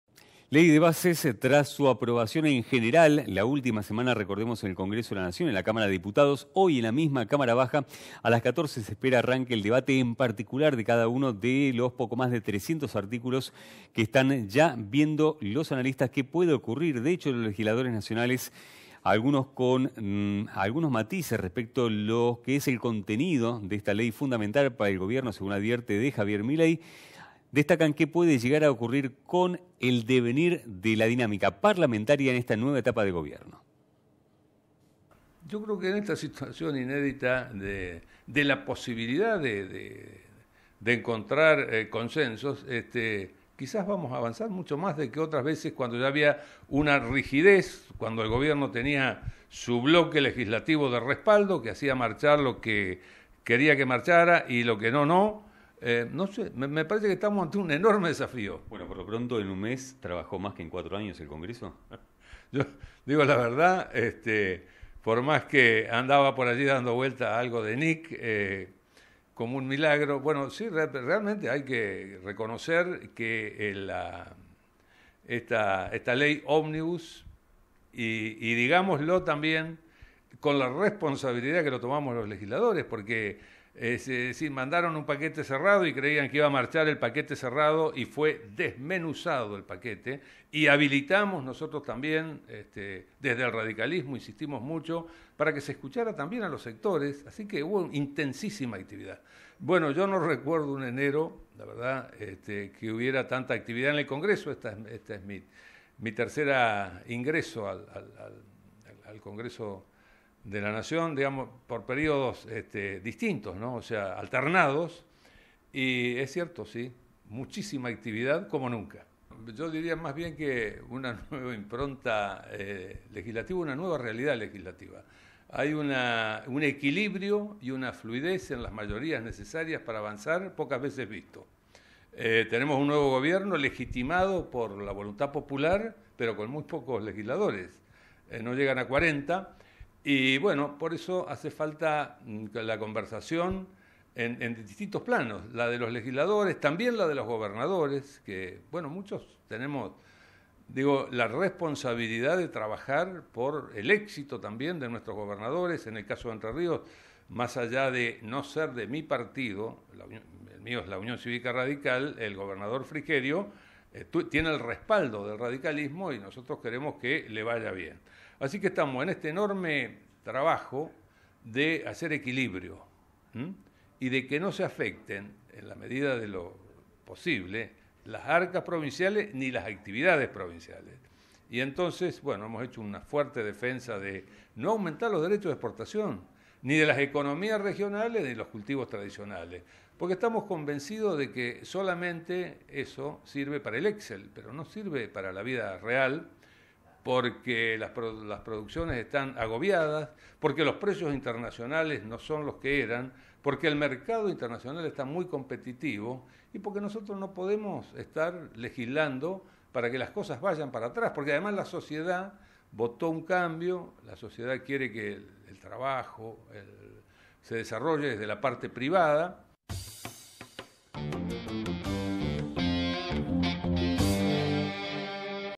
Ley de Bases: Empieza el debate en particular de los artículos
A las 14 hs se espera que dé comienzo en Diputados el tratamiento de la iniciativa oficial, que ya fue aprobada en general en dicho recinto la semana pasada. Legisladores Nacionales y consultores resaltan que a nivel parlamentario en un mes se trabajó más que en los últimos años y destacan el buen funcionamiento de las instituciones. Expectativas por lo que ocurra luego eventualmente en el Senado, ya que cualquier modificación que se introdujera haría retrotraer toda la gestión a la Cámara Baja